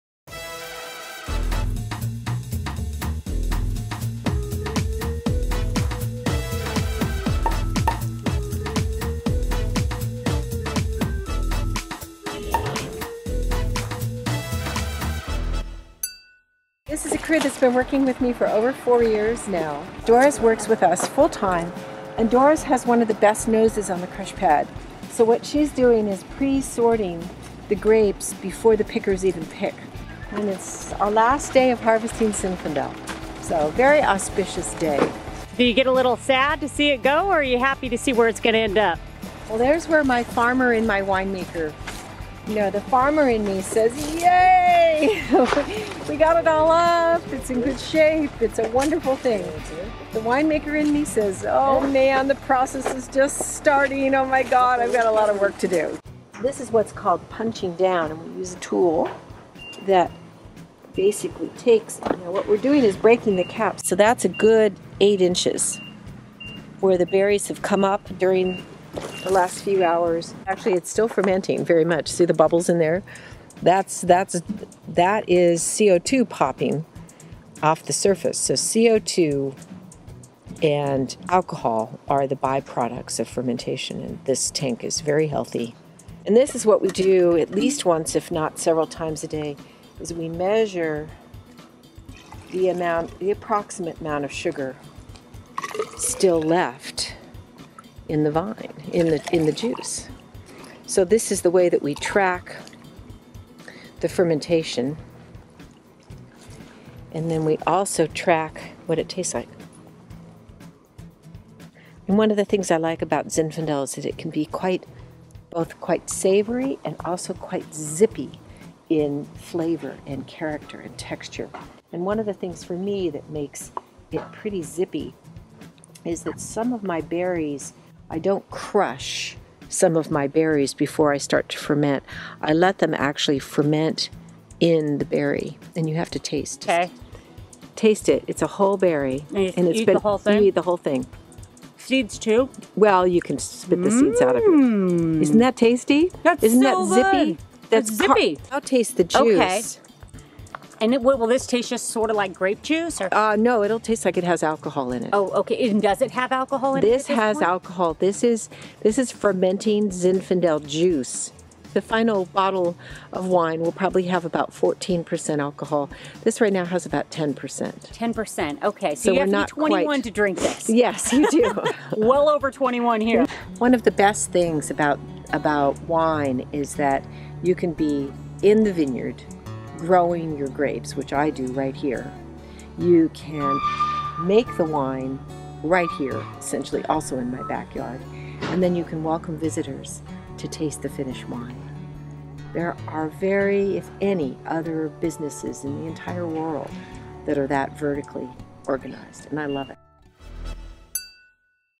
Learn about Making Wine with Winemaker Julie Johnson: Harvest & Punch Down - Wine Oh TV
Watch Wine Oh TV with Monique Soltani and learn how to make wine with one of Napa Valley's most famous winemakers.

Winemaking 101 with Julie Johnson: Harvest & Punch Down